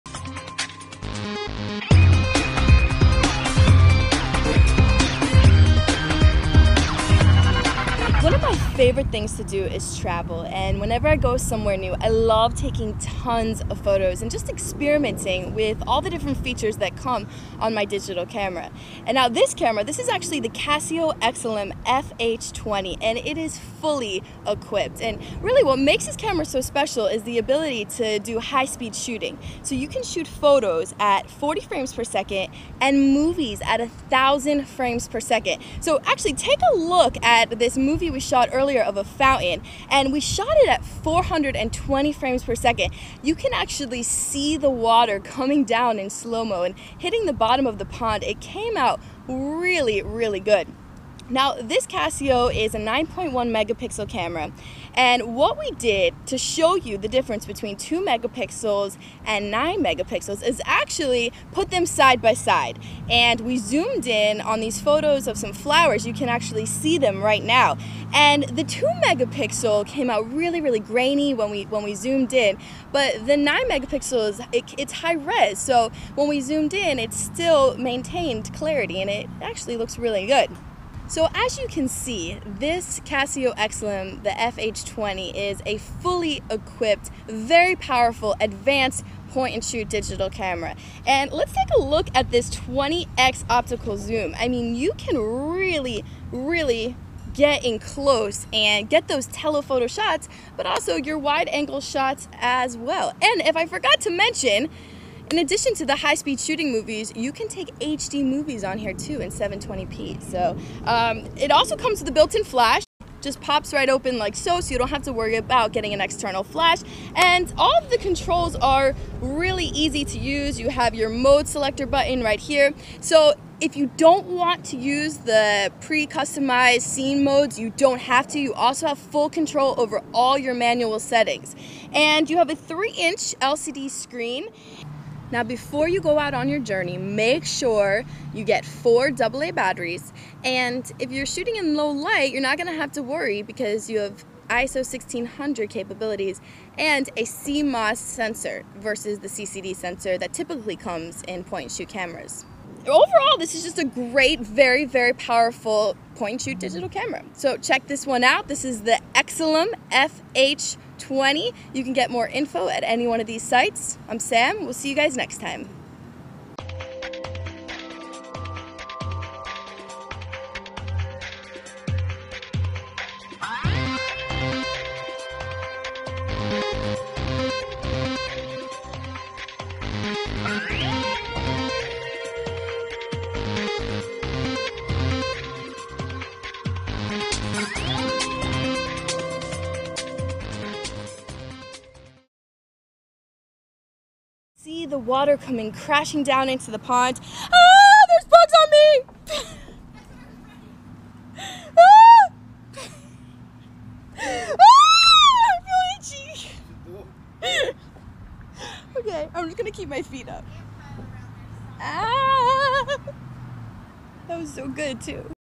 Casio EX-FH20BK Digital SLR Camera
Casio's new EX-FH20 EXILIM digital camera is great for ultra-fast picture recording, and Sam is here to tell you why in this video demonstration.  Sam shows how the Casio EX-FH20 takes 40 photos per second and video up to 1000 frames per second.

Check out the great shot of the waterfall Sam shot to see how the detail in the cascading water is retained in this very short glimpses into time.

The Casio EX-FH20 Digital Camera's 20x zoom makes it the ideal camera for sports and outdoor photography. In addition to beautiful still images, the Casio EX-FH20 Digital Camera is capable of capturing slow-motion video at up to 1,000 fps.